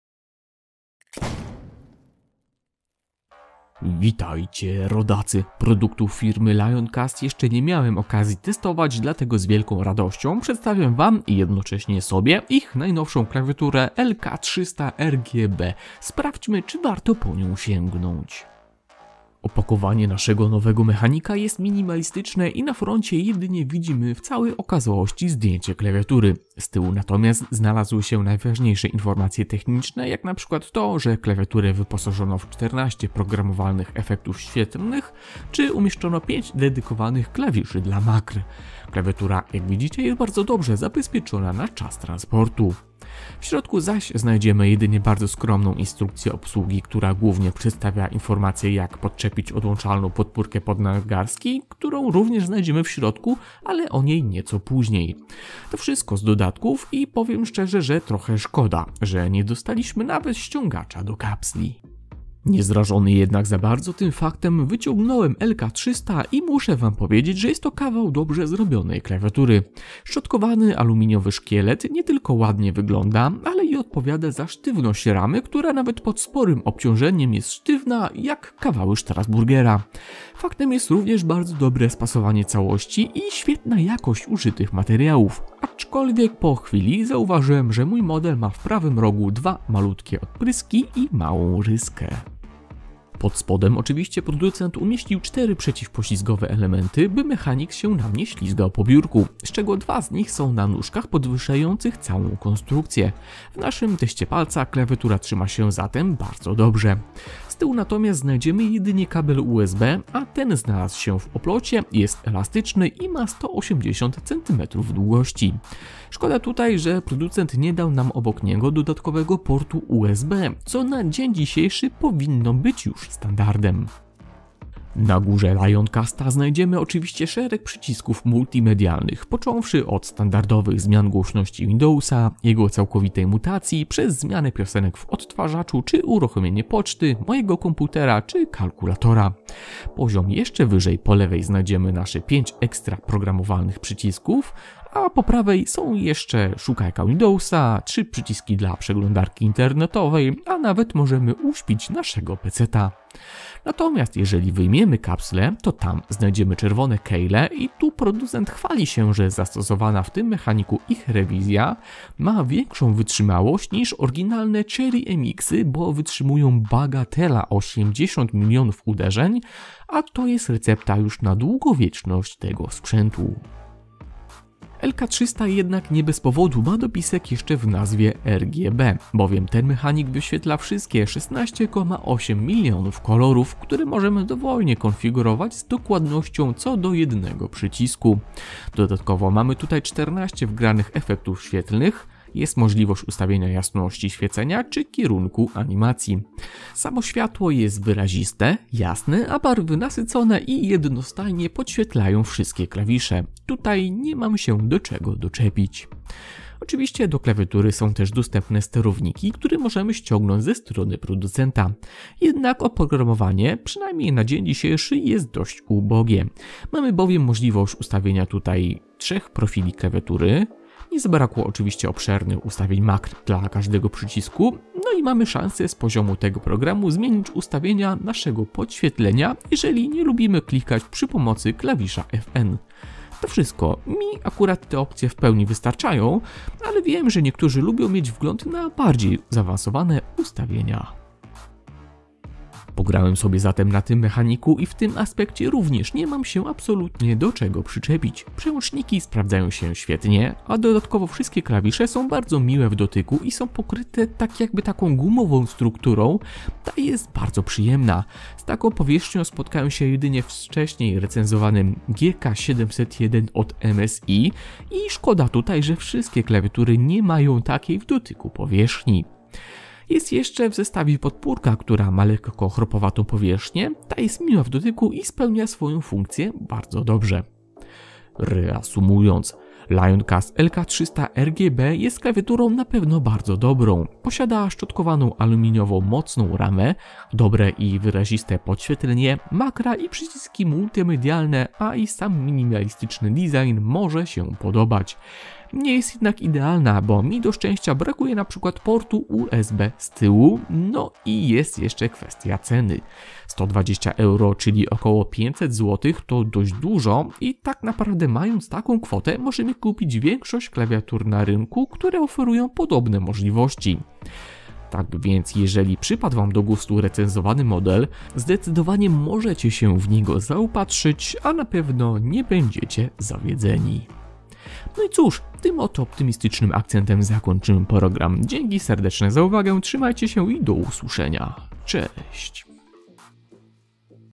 Lioncast LK300 RGB - Recenzja 4K
Test klawiatury dla graczy LK300 RGB - czy ten sprzęt moze miec jakies wady?

Pytania, napisz komentarz - postaram sie odpisać.